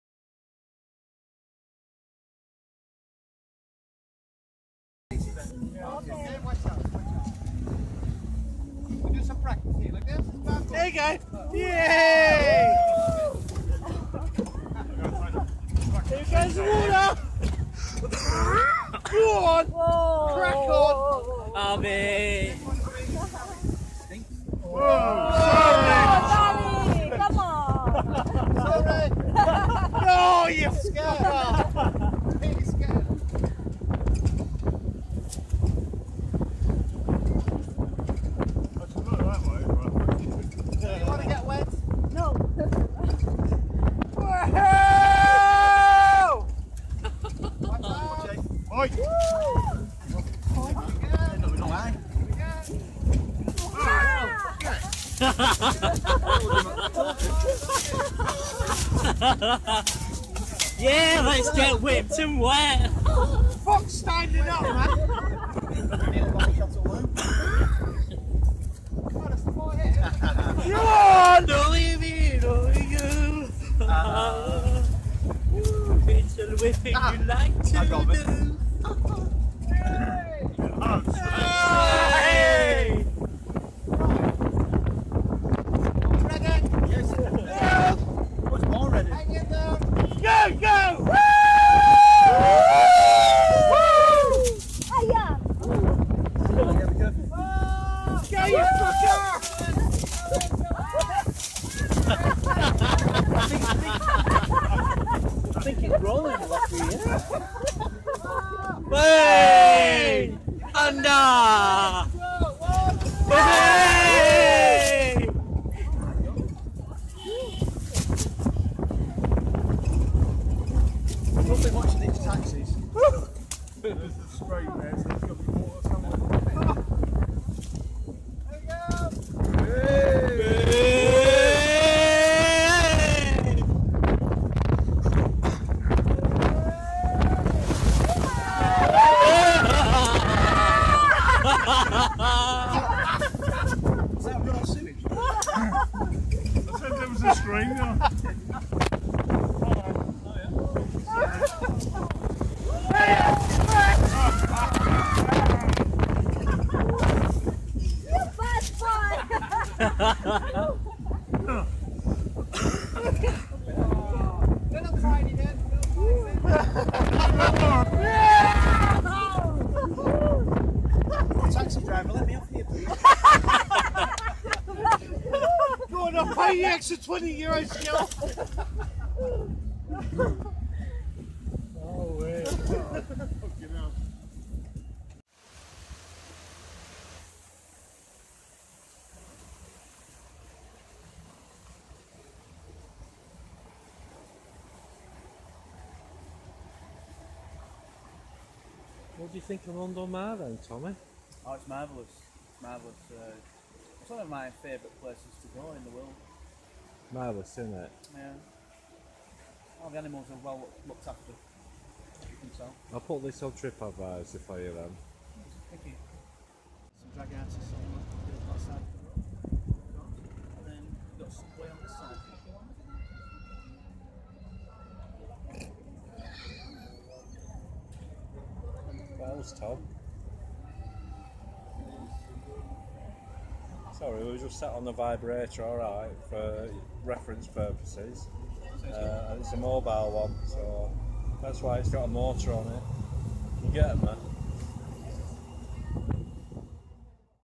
Benidorm 2022 Preview
A few snippets of past activities and excursions that we've been on. We aim to be doing these again, with different cameras and also a few new excursions too. All will be revealed.